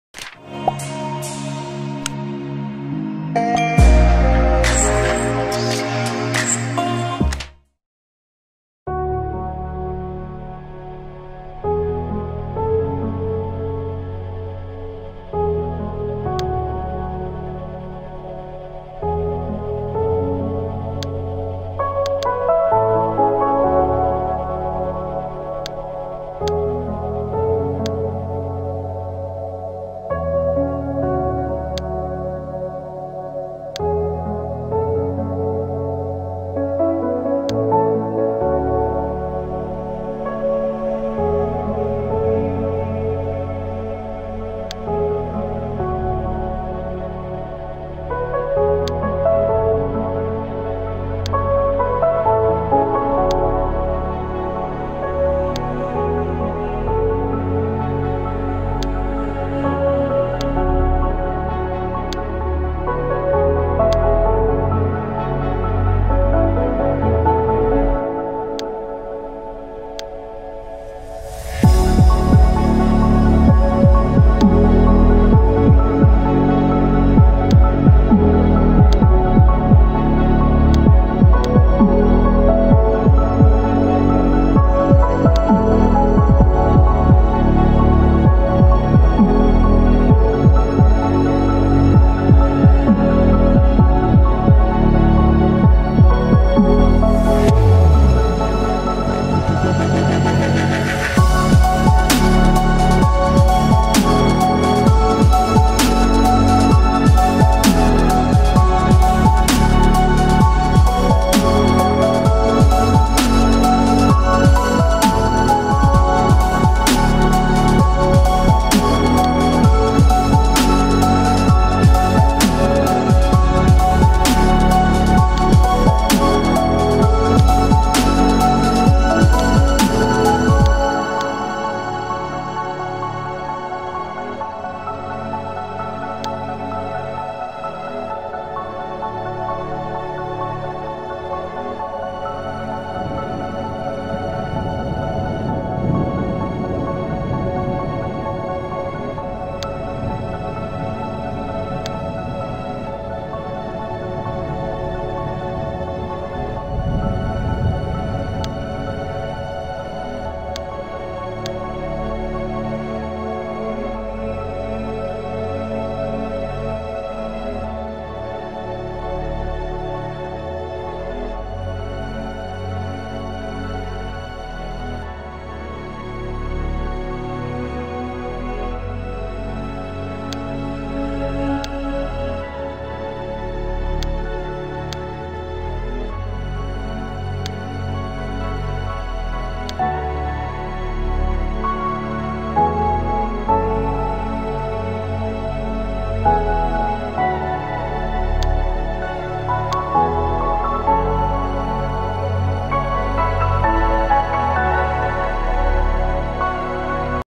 HOW TO USE CLOTHWORKS IN SKETCHUP 2020
This video contains clothwork simulation for SketchUp, It is extension use in SketchUp for cloth simulation. Also, this video shows how to apply texture for UV mapping on fabric.

Enjoy the full video and make sure to give your feedback in the comment section.

Open to collaborate...